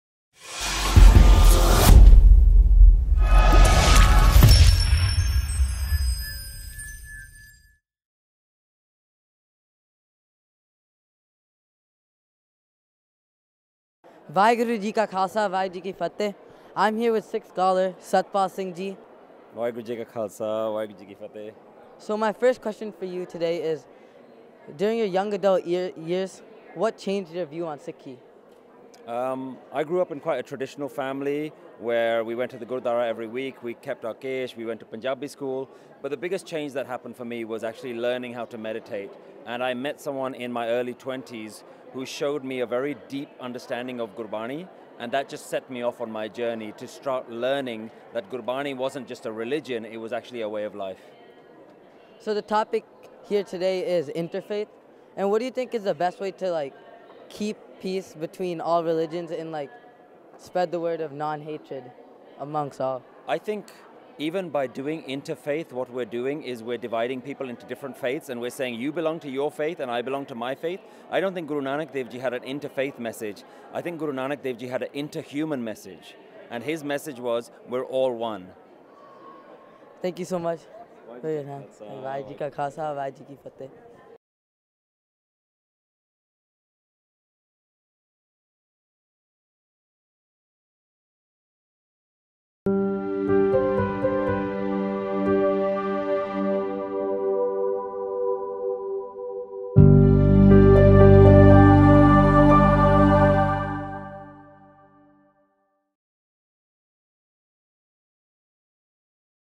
Interview with Satpal Singh (Humanitarian)
At Oneness - A Light for Humanity | Guru Nanak Dev Ji's 550th Birth Anniversary
Let's Share a Meal & Sikh Chamber of Commerce Global Initiative.

Bhai Satpal Singh has delivered lectures and meditations classes for over ten years. He combines a unique blend of ancient spiritual wisdom with easy-to-follow techniques that bring value to all who attend.